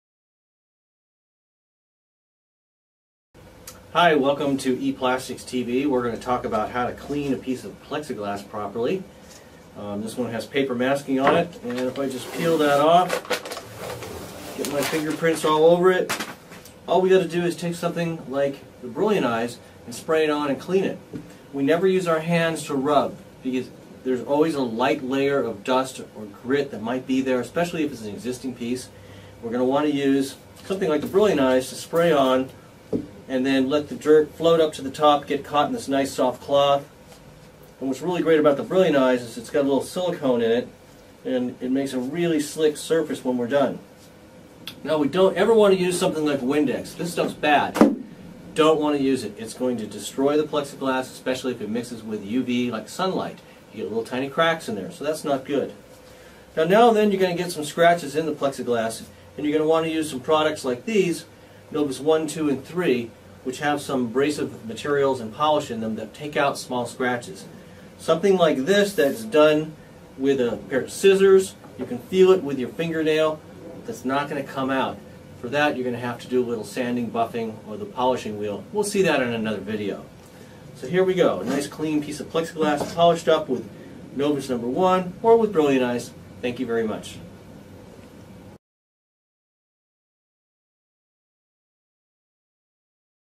ePlastics | How to Clean Plexiglass
This video shows how acrylic (aka Plexiglas®, Perspex®, Lucite®, etc.) is cleaned without scratching the surface.

Providing plastics solutions since 1914.

“When Others Can’t ... We Can!”